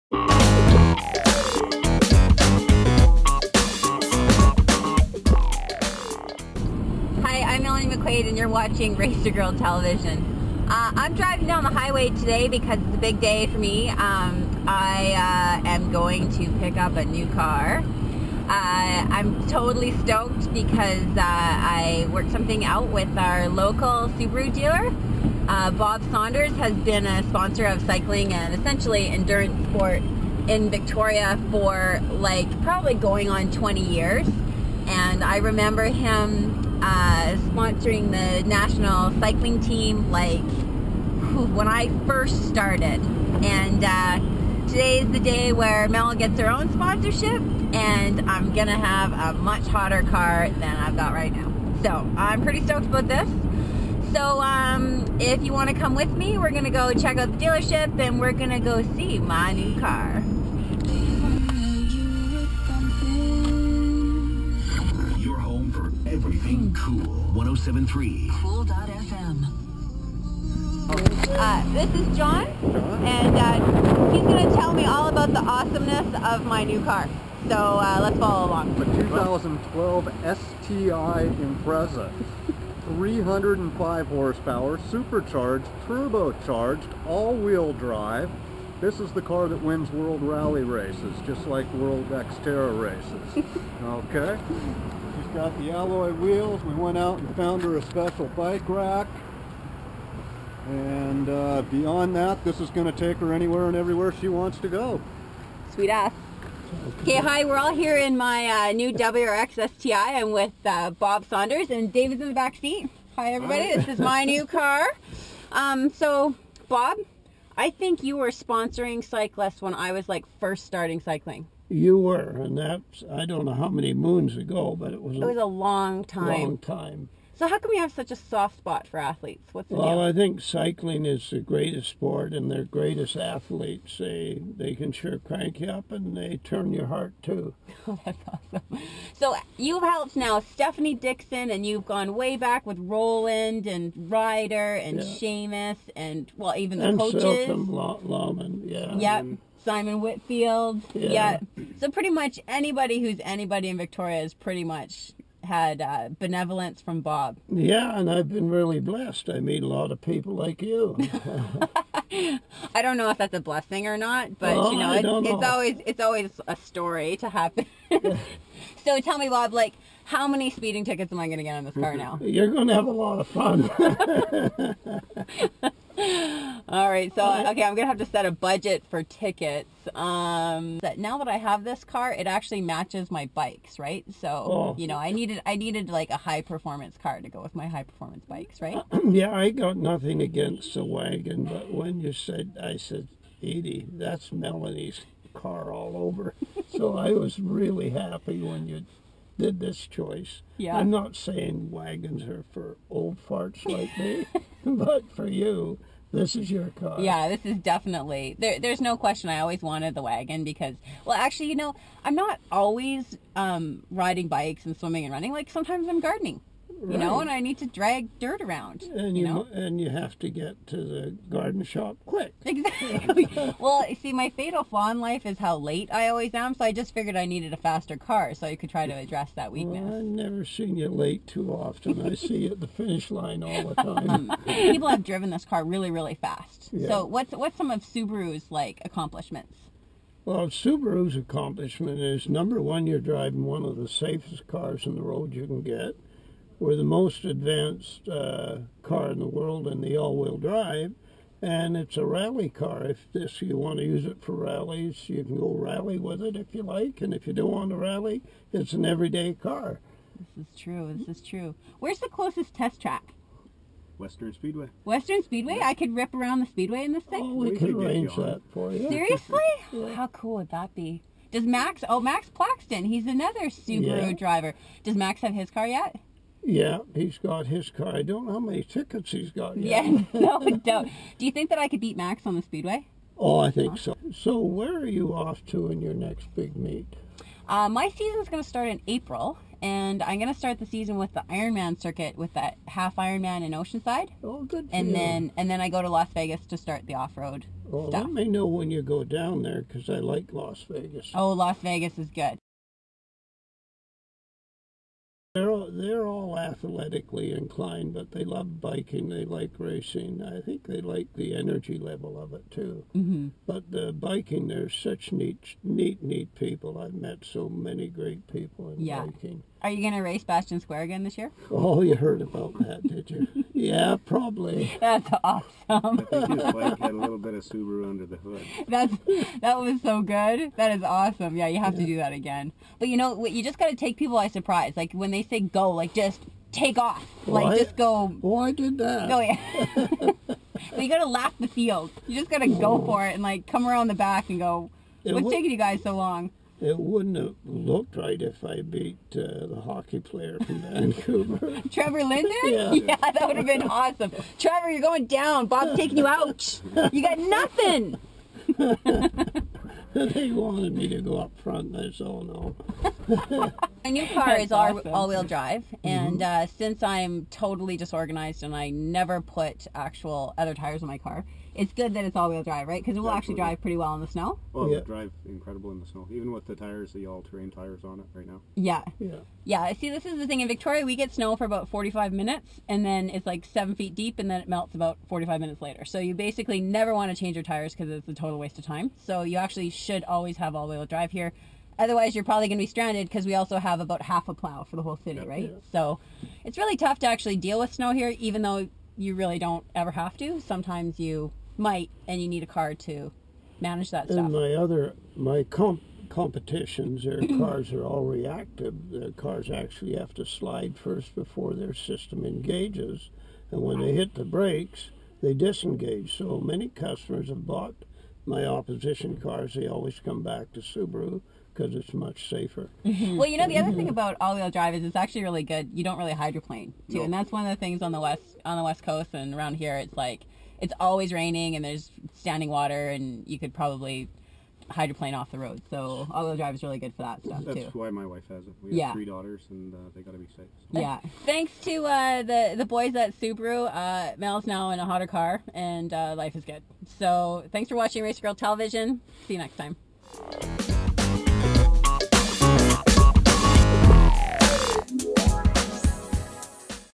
Racergirl Television Episode 15: Fast Cars Are Awesome
I get to roll away from Saunders Subaru in a WRX Sti.  I have a fun little chat with Bob about fast cars, AWD and destroying Trevor Linden at the Bastion Square Grand Prix.

Big props to Saunders for the 20 + years they have been contributing locally to Olympic and professional athletes' success around the world.